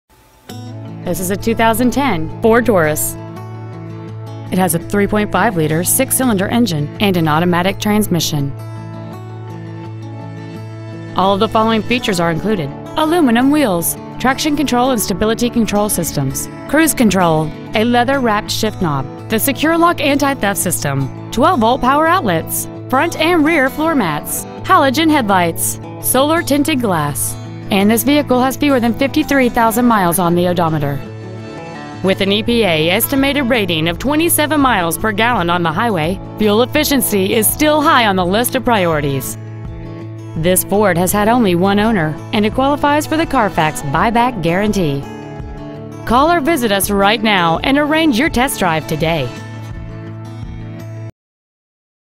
Pre-Owned 2010 Ford Taurus Beaufort SC 29906
Year : 2010
 Make : Ford
 Model : Taurus
 Engine : 3.5L V6 Duratec engine
 Trans . : Automatic
 Exterior : Silver
 Miles : 52,390
 Interior : Other